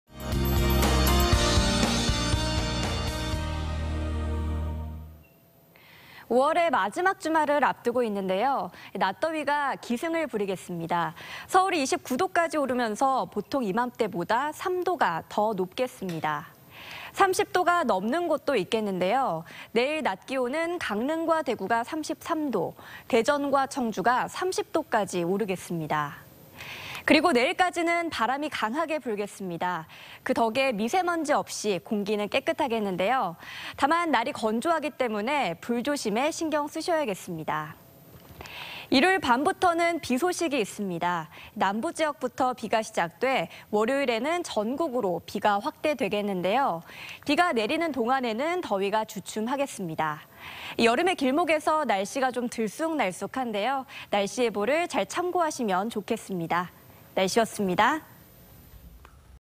[날씨] 주말 더위 기승…토요일 서울 낮 29도
5월의 마지막 주말을 앞두고 있는데요, 낮 더위가 다시 기승을 부리겠습니다. 서울이 29도까지 오르면서 보통 이맘때보다 3도가 더 높겠습니다.

30도가 넘는 곳도 있겠는데요, 내일 낮 기온은 강릉과 대구가 33도, 청주와 대전은 30도까지 오르겠습니다.

그리고 내일까지는 바람이 강하게 불겠습니다. 그 덕에 미세먼지 없이 공기는 깨끗하겠는데요, 다만 날이 건조하기 때문에 불조심에 신경쓰셔야겠습니다.

일요일 밤부터는 비 소식이 있습니다. 남부 지역부터 비가 시작돼 월요일에는 전국으로 확대되겠는데요,

비가 내리는 동안에는 더위가 주춤하겠습니다. 여름의 길목에서 날씨가 좀 들쑥날쑥 한데요, 날씨 예보를 잘 참고하시면 좋겠습니다.